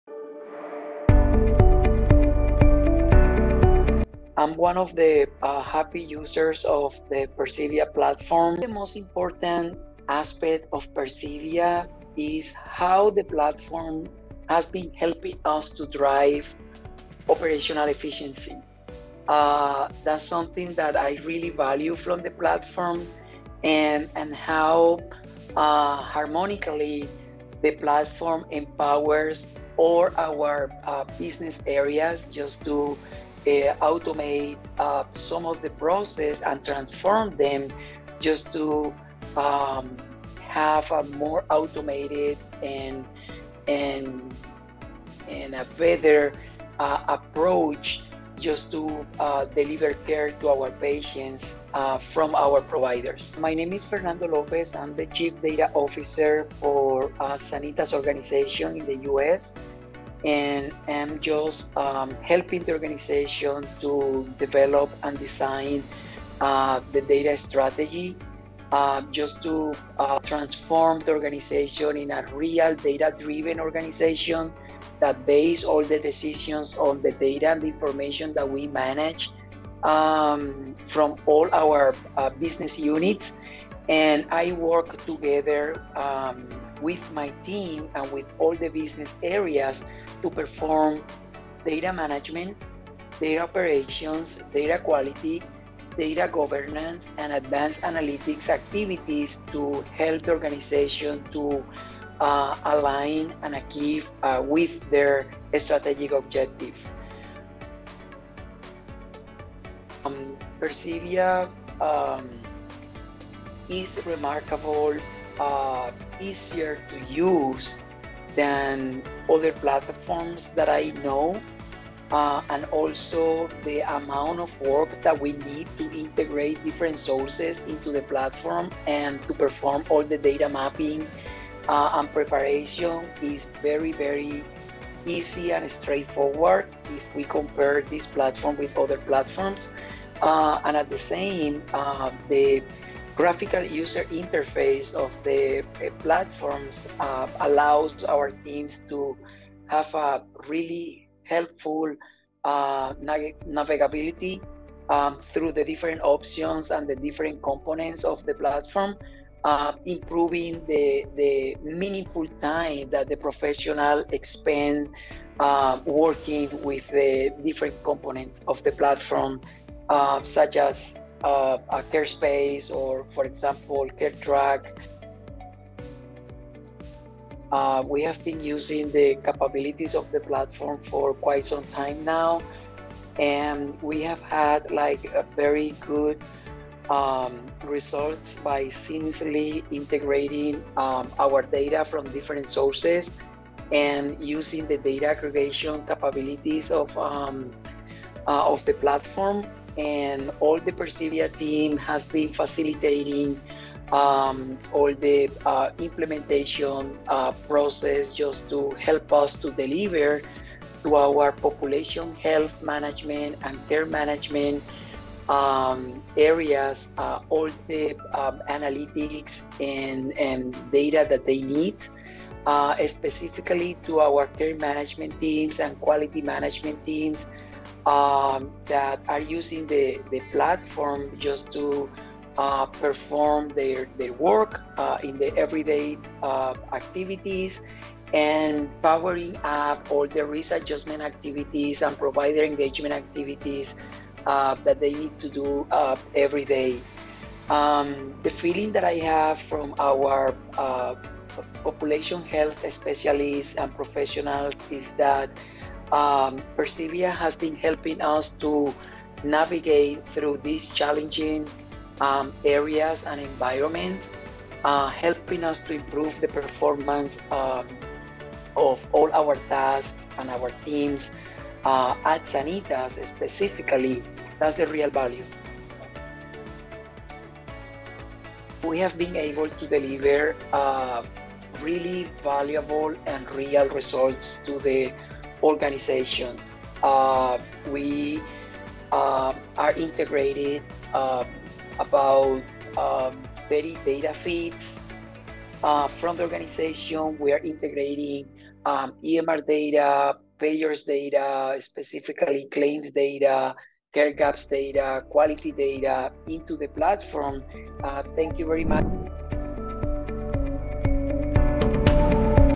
Sanitas Journey with Persivia | Fernando Lopez | Customer Story
Hear from Fernando Lopez, Chief Data Officer at Sanitas, as he shares how Persivia's robust data management and integration capabilities successfully addressed their most significant challenges—challenges that remained unresolved by any other solution on the market.